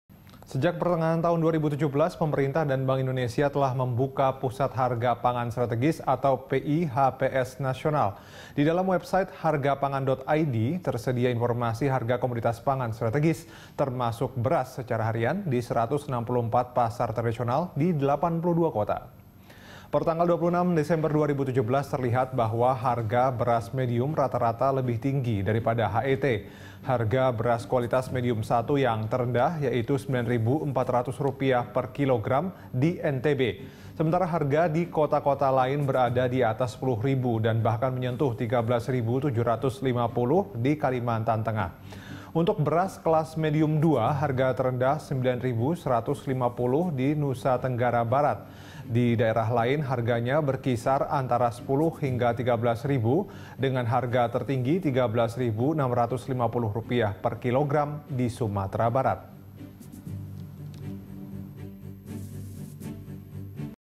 Daftar Harga Beras Medium Per 26 Desember 2017
Sejak pertengahan 2017, pemerintah dan Bank Indonesia telah membuka pusat harga pangan strategis (PIHPS) Nasional. Di dalam website hargapangan.id tersedia informasi harga komoditas pangan strategis termasuk beras secara harian di seratus 64 pasar tradisional di 82 kota.

Facebook.com/BeritaSatuTV
Youtube.com/BeritaSatu